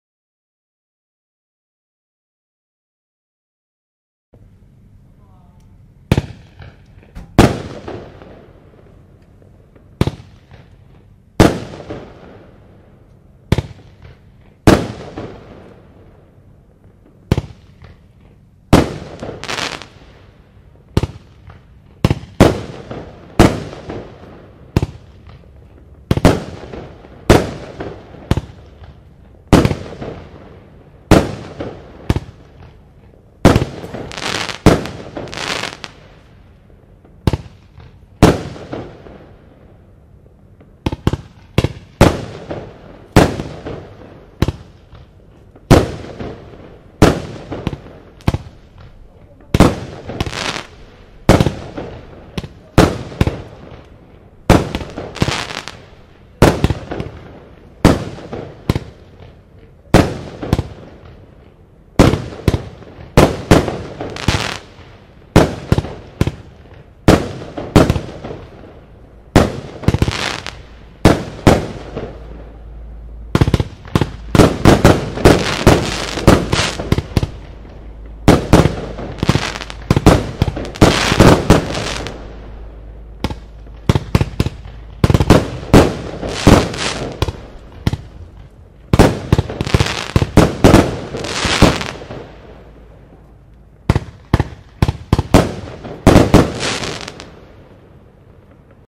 Wicked Machine - 02487 | CAT F2 | Official video
Wicked Machine | Fireworks | Feuerwerk | Vuurwerk

You are looking at the product: Wicked Machine. This fireworks has the article number 02487 and is part of the Showboxes fireworks collection. The powderweight of the Wicked Machine is 596 gram. The type of this product is: Compound fireworks in the Category CAT F2.

Enjoy the show! Go to our website to find a dealer near you to buy the Wicked Machine.